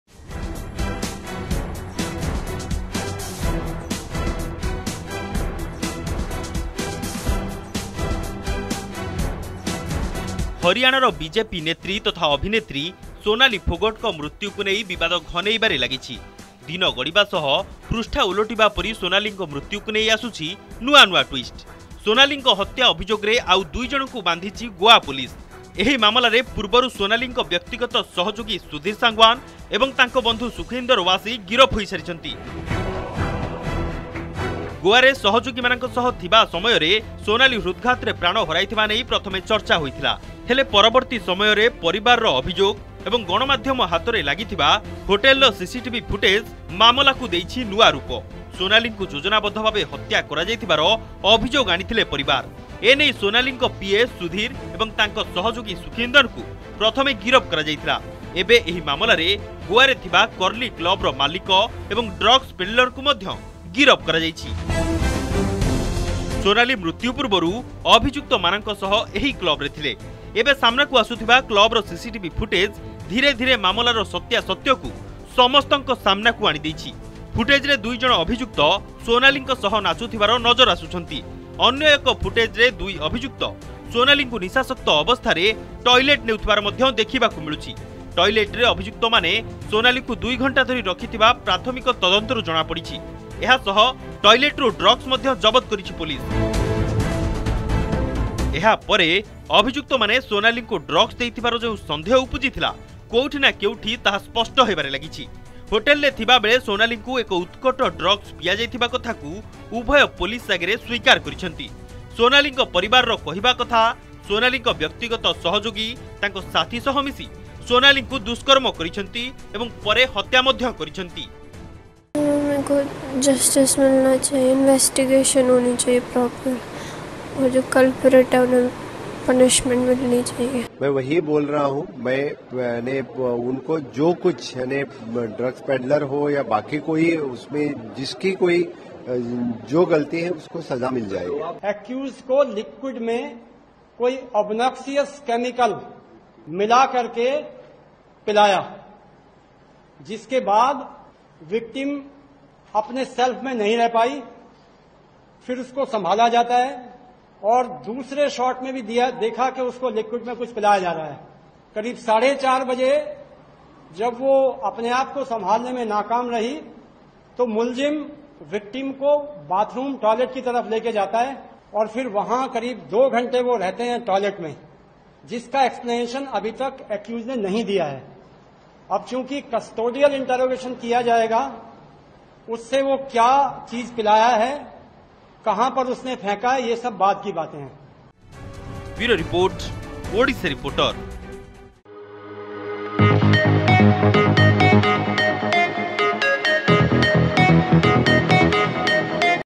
ରାତିରେ ସୋନାଲୀଙ୍କ ସହ କଣ ହୋଇଥିଲା | Odisha Reporter
ହରିୟାଣାର ବିଜେପି ନେତ୍ରୀ ତଥା ଅଭିନେତ୍ରୀ ସୋନାଲି ଫୋଗଟଙ୍କ ମୃତ୍ୟୁକୁ ନେଇ ବିବାଦ ଘନେଇବାରେ ଲାଗିଛି । ଦିନ ଗଡ଼ିବା ସହ ପୃଷ୍ଠା ଓଲଟିବା ପରି ସୋନାଲିଙ୍କ ମୃତ୍ୟୁକୁ ନେଇ ଆସୁଛି ନୂଆ ନୂଆ ଟ୍ୱିଷ୍ଟ । ସୋନଲିଙ୍କ ହତ୍ୟା ଅଭିଯୋଗରେ ଆଉ ଦୁଇଜଣଙ୍କୁ ବାନ୍ଧିଛି ଗୋଆ ପୋଲିସ ।